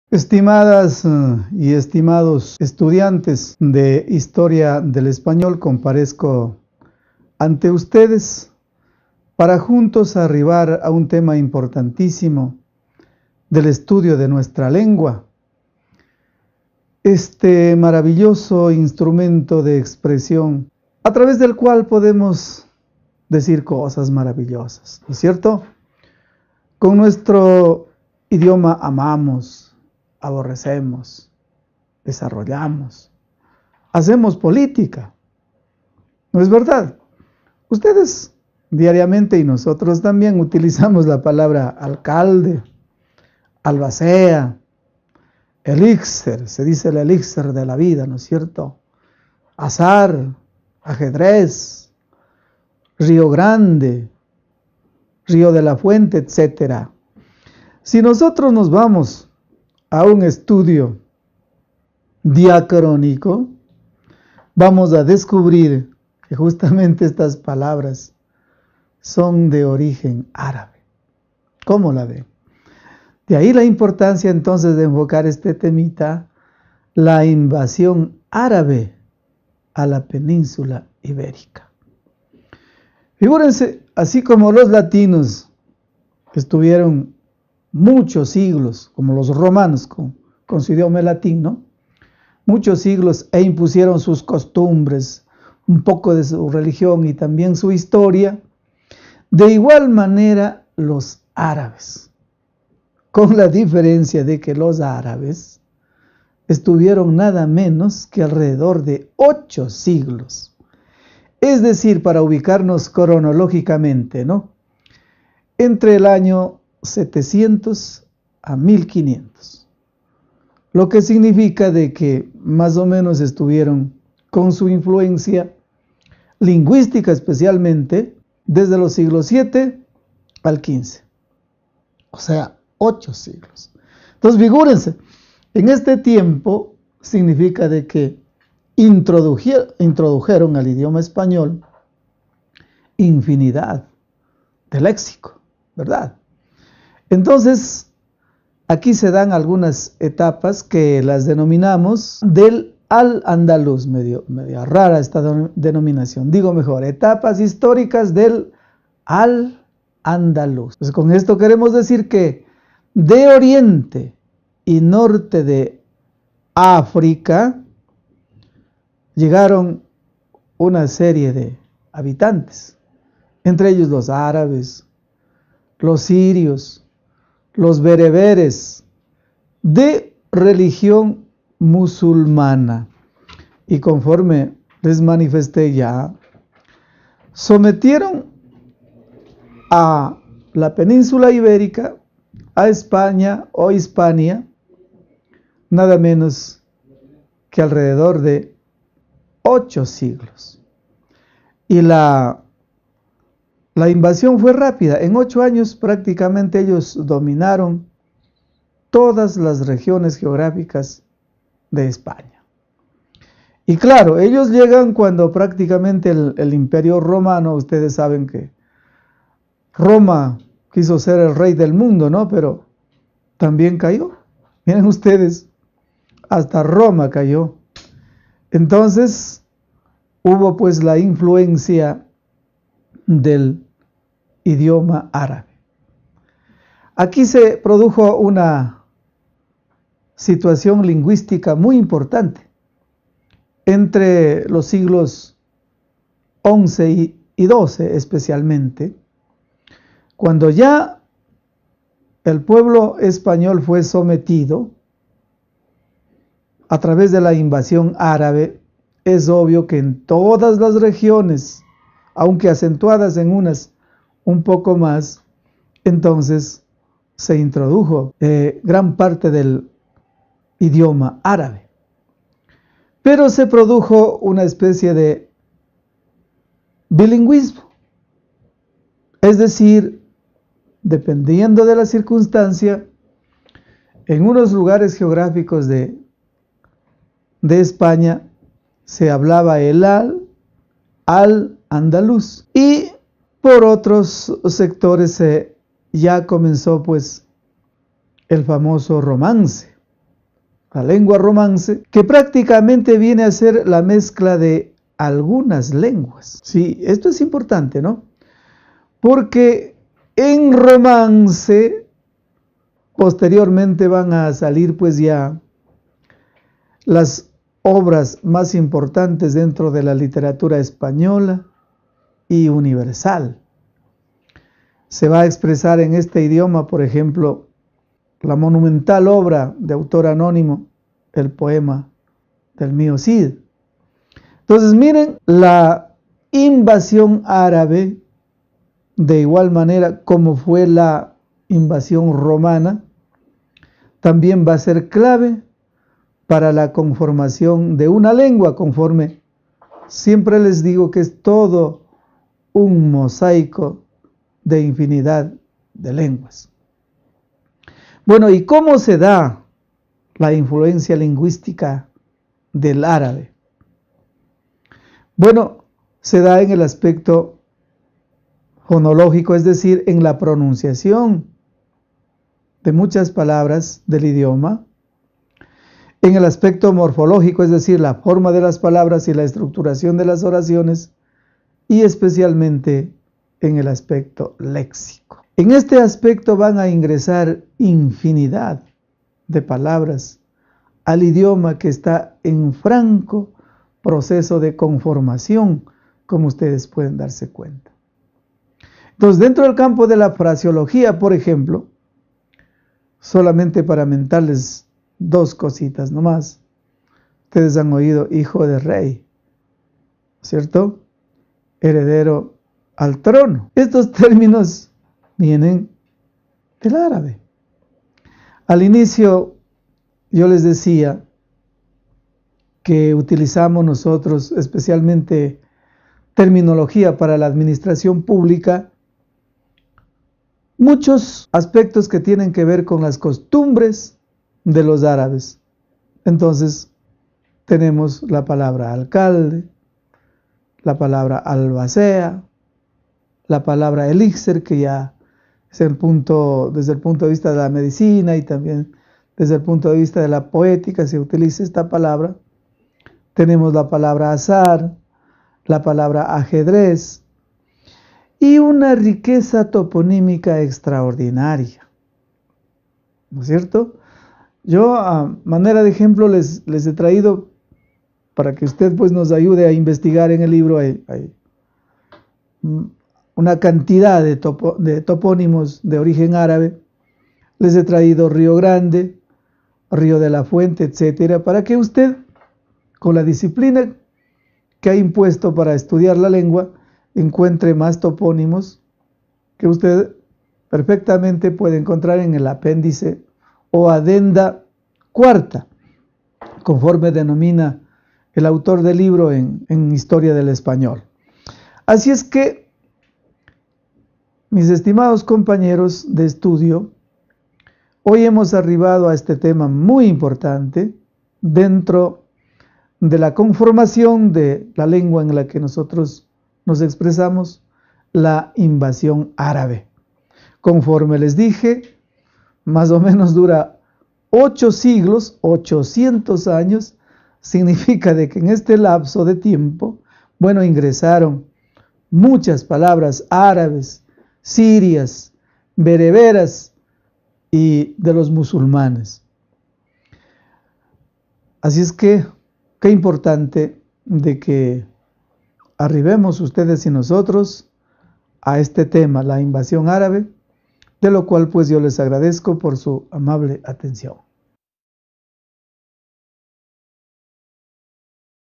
UTPL LA INVASIÓN ARABE [(CCEE - LENGUA Y LITERATURA)(HISTORIA DEL ESPAÑOL)]
CCEE - Lengua y Literatura
Historia del Español
Tema: Invasión Arabe
Ponente: Luis Antonio Quizhpe